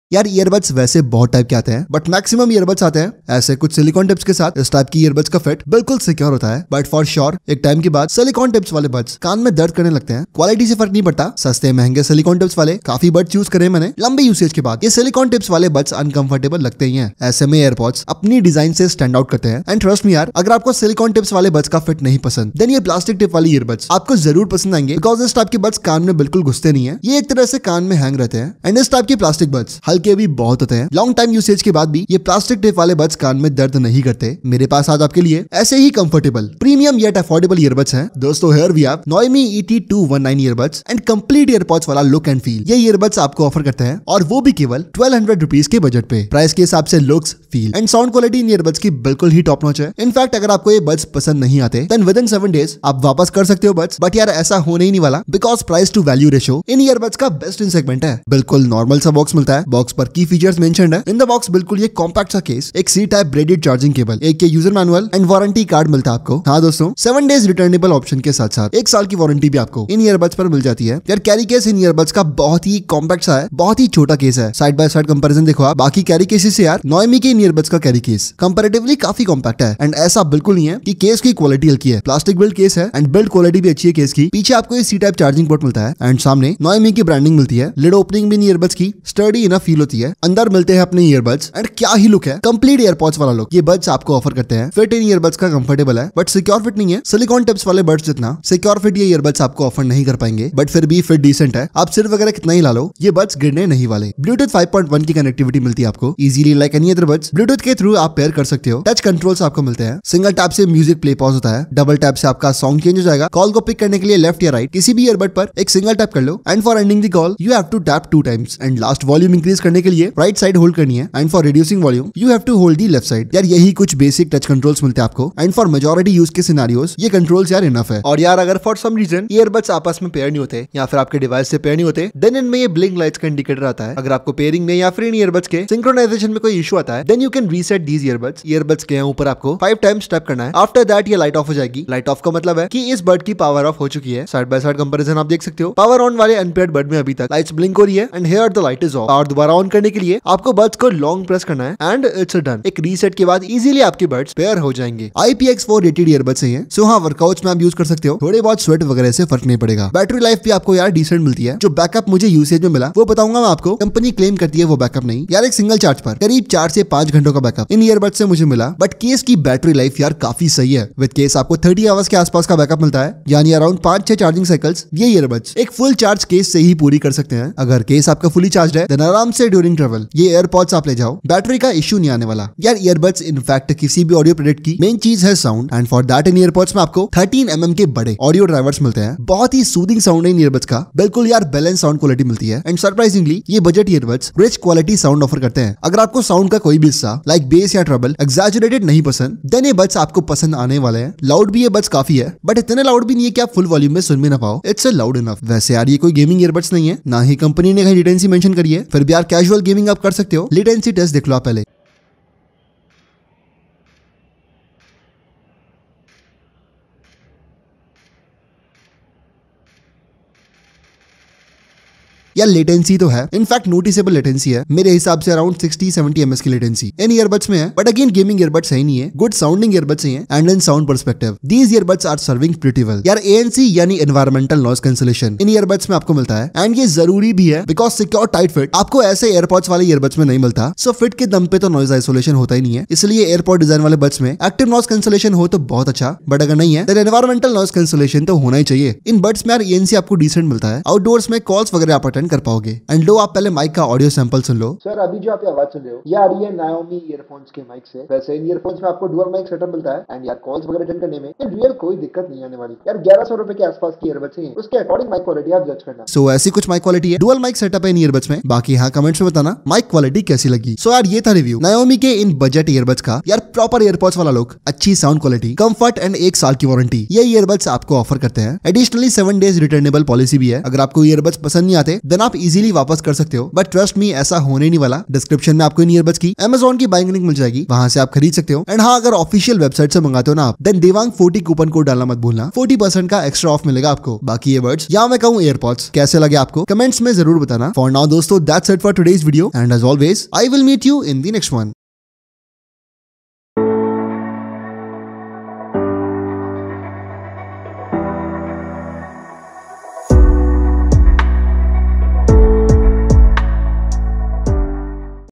Apple airpods under 1500 rs | Noymi Et 219 TWS Earbuds
Nyomi ET 219 Tws earbuds -

My Gears -
Microphone - 1.
Laptop - 1.
Gaming Device
Tablet \ ipad
Headphone - 1.

Video Highlights -
0:00 Introduction
0:58 Unboxing
1:11 Build quality
1:44 Connectivity and Features
2:39 Battery backup
3:02 Audio quality
3:25 Gaming experience\latency test
3:53 Mic quality
4:31 Conclusion